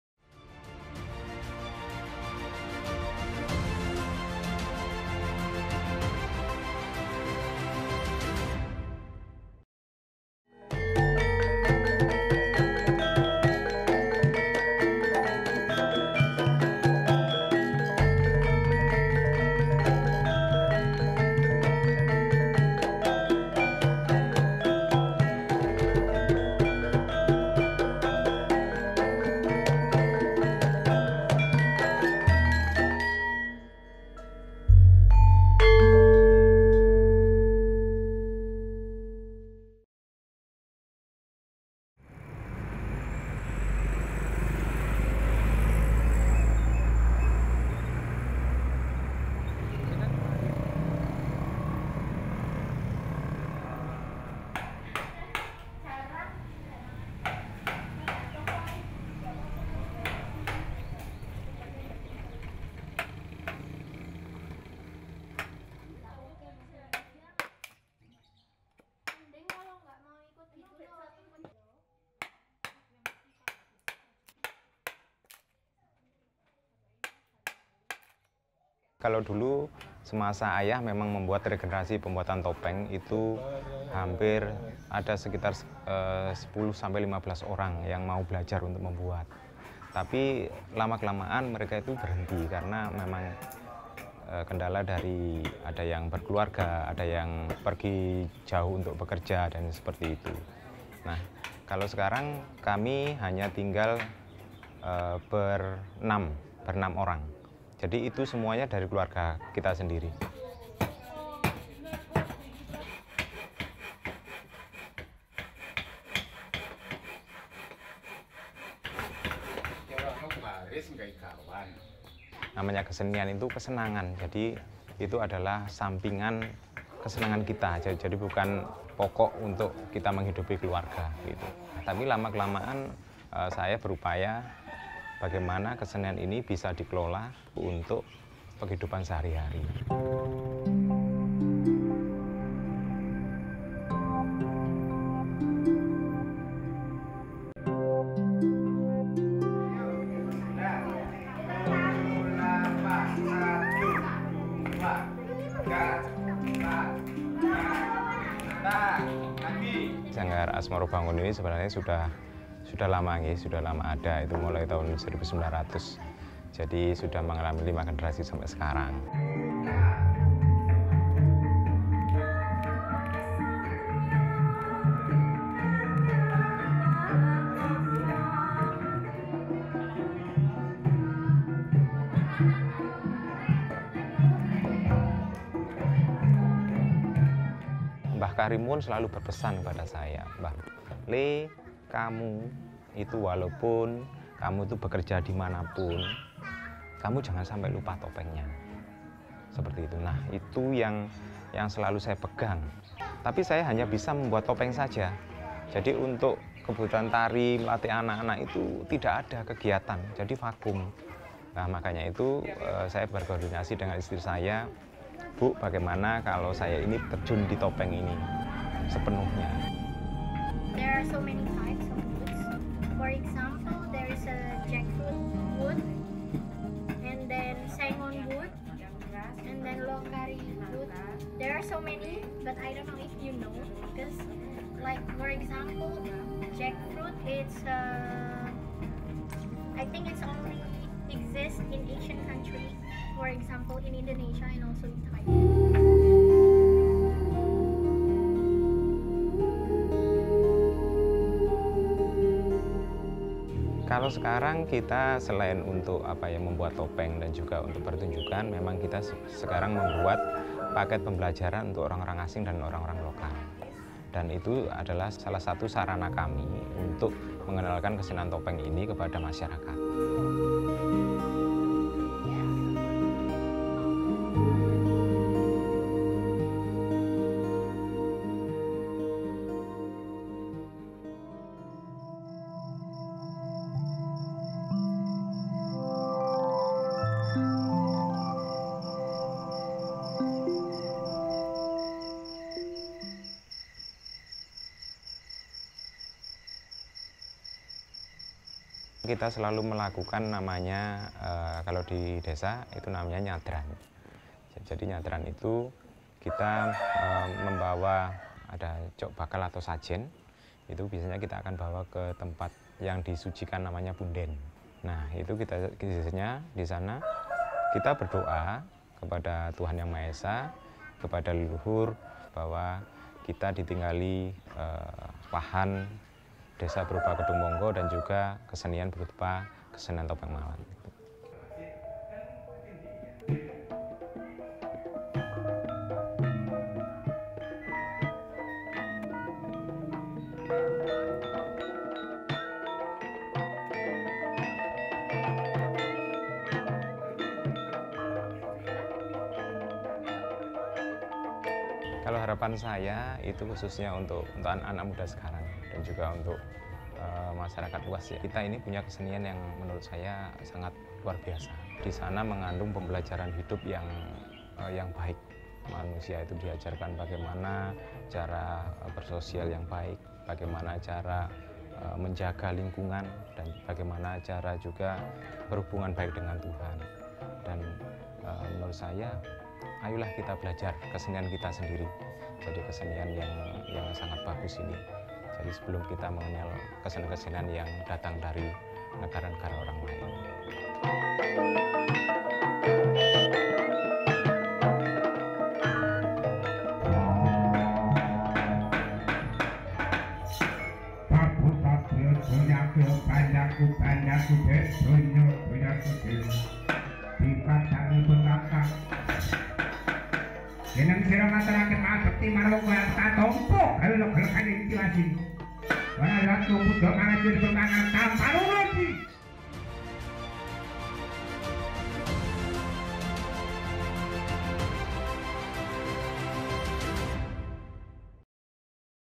EAGLE WEB SERIES: TOPENG MALANGAN
Topeng Malangan adalah suatu kesenian tradisional asal Malang, Jawa Timur. Namun di tengah arus modernisasi dan tuntutan ekonomi kini satu persatu pengrajin topeng tersebut telah hilang. Lantas bagaimana para seniman Topeng Malangan yang masih ada hingga saat ini berupaya mengenalkan seni dan tradisi mereka pada generasi muda?

Simak kisahnya dalam Eagle Web Series episode Topeng Malangan